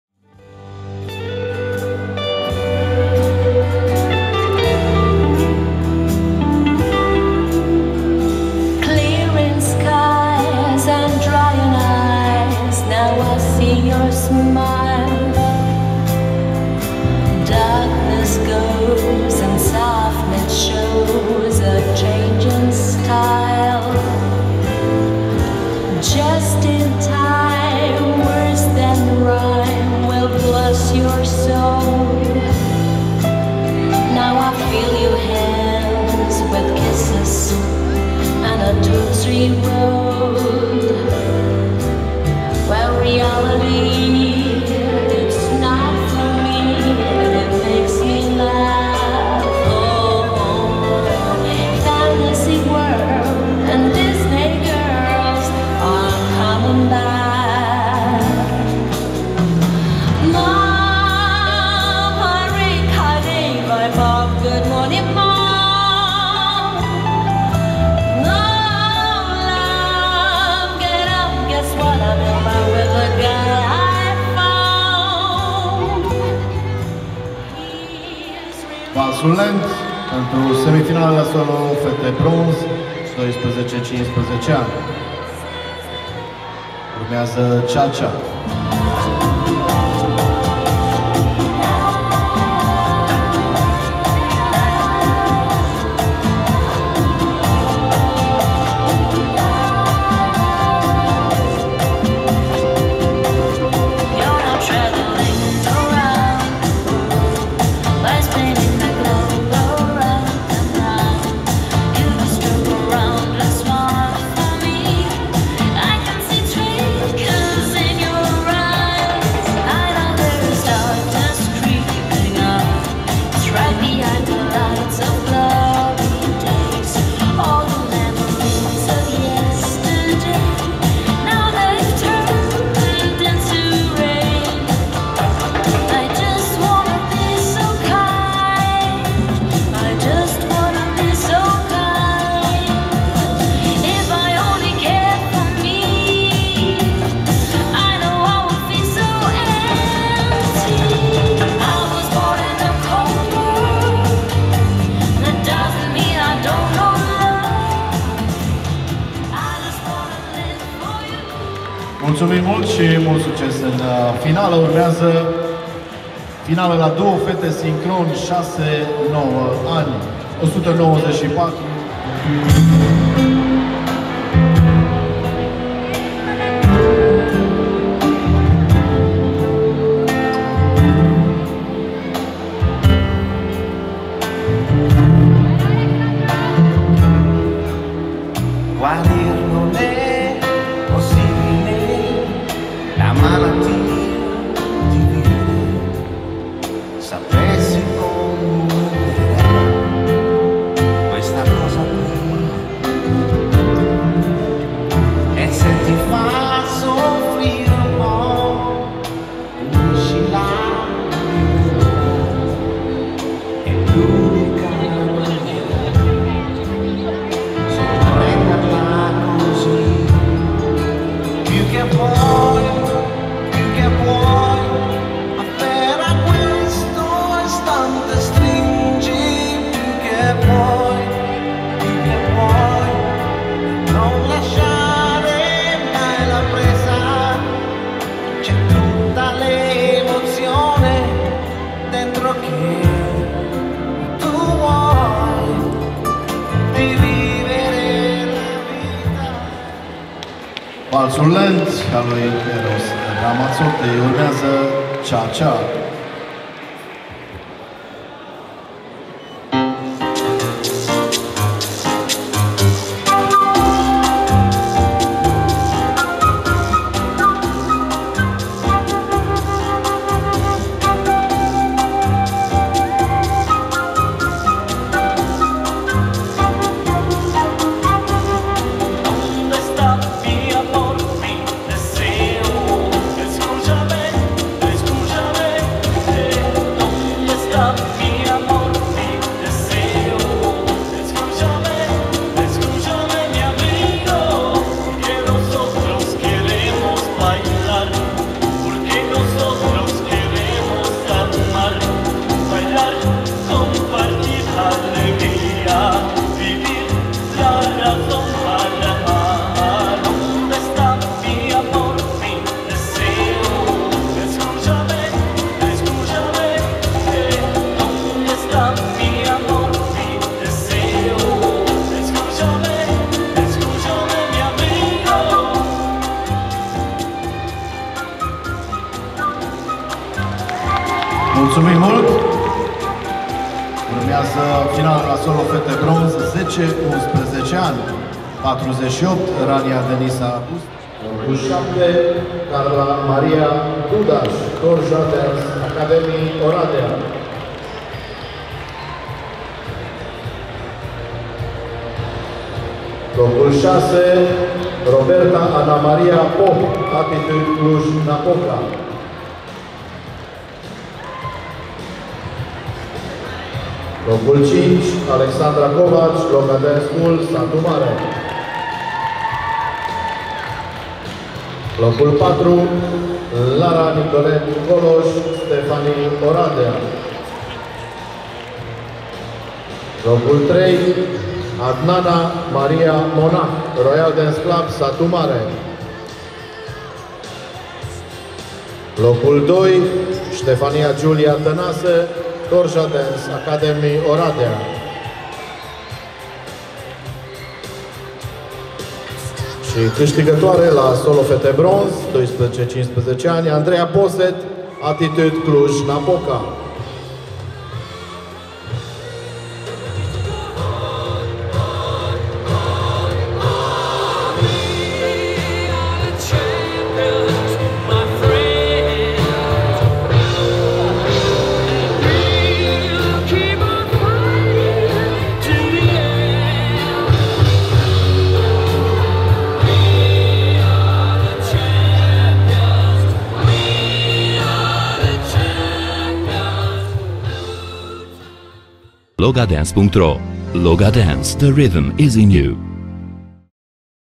Loga Dance School la Cupa Satmarului - Solo Fete Bronz (12-15 ani) Ballroom Dancing
Loga Dance School la Cupa Satmarului

Concurs organizat in Satu Mare, Jud. Satu Mare (Romania)

16 Septembrie 2023

Semifinala & Finala.

Kovacs Alexandra
Locul 5 din 9 fete
Clasa: Solo Fete Bronz
Categoria: 12-15 ani
Sectiunea: Latino
Dansuri: Cha Cha

Felicitari sportivilor pentru rezultatul obtinut!